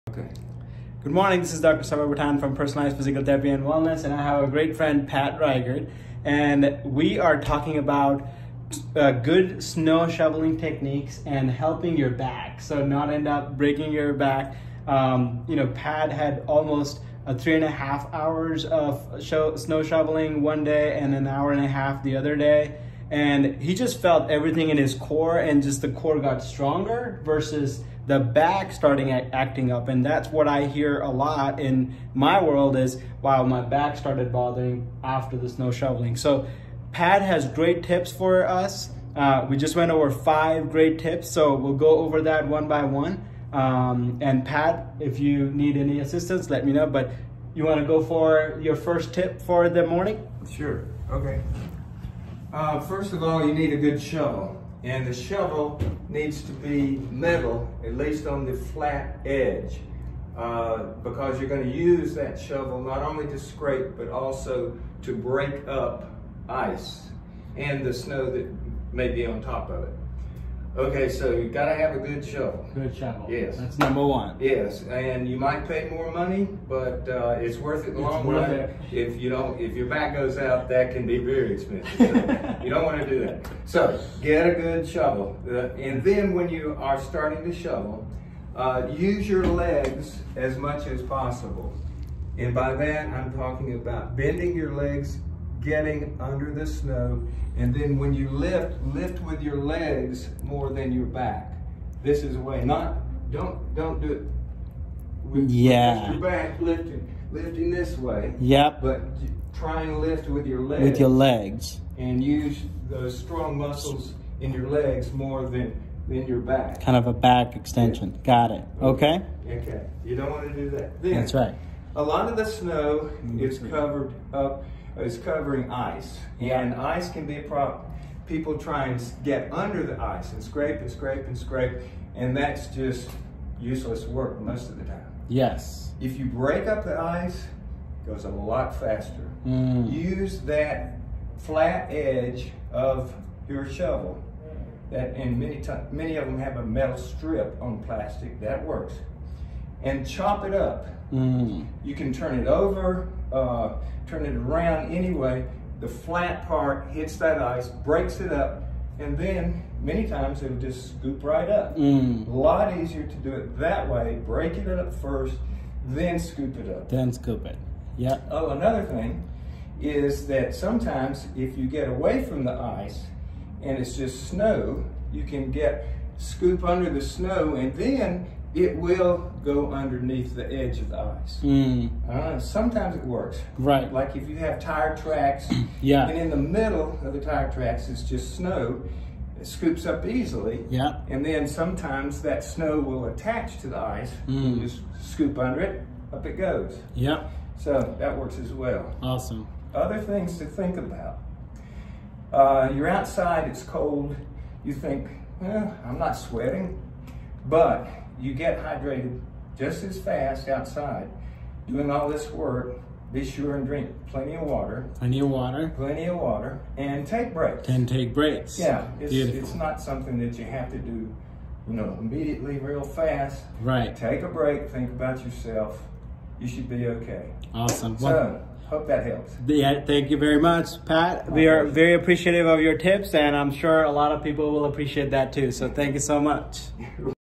5 Snow shoveling tips to prevent injuries.
Pat Reighard, an experienced snow shoveler goes on video with Dr Sarvar Pathan to talk about

1. How not to shovel the snow

2. How to avoid having “Back pain” and other body injuries

3. What has he done while needing the clean his, his mom’s and more driveways over the past few days

4. Some general physical fitness tips to avoid medical complications and health problems.

Listen up with Dr Sarvar Pathan, with Personalized physical therapy and wellness.